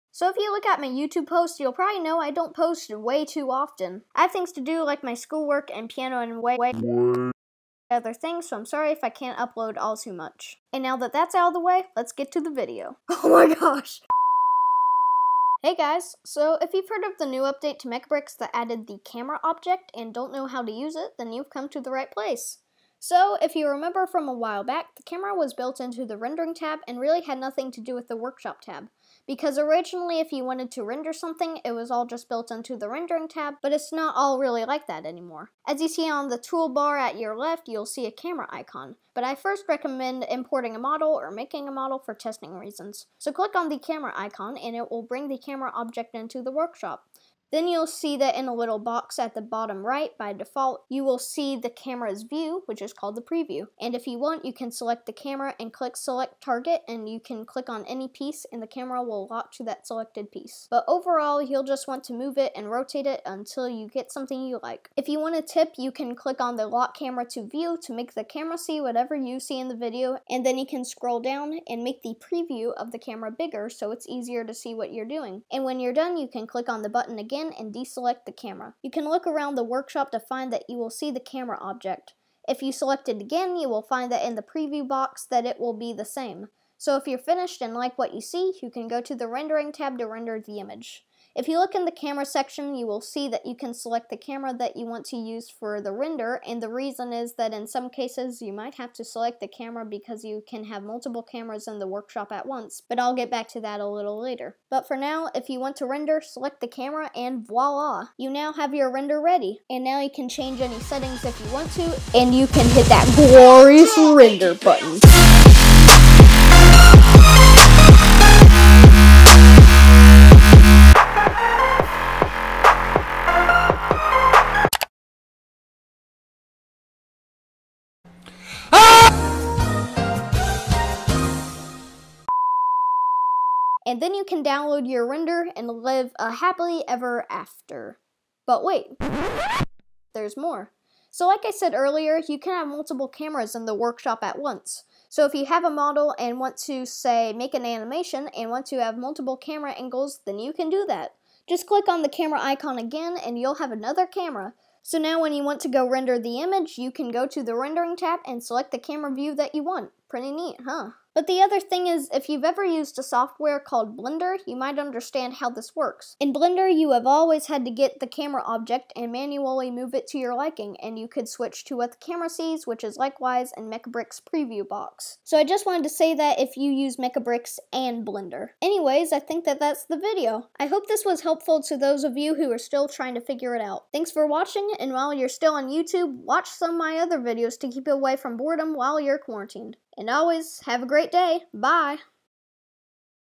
How to Use The New Mecabricks Camera Object
Hey guys! Today I will show you how to use the new Camera Object in
I hope this was helpful, and have a God Blessed Day!

Byyeeeee!

~Lego_Lover~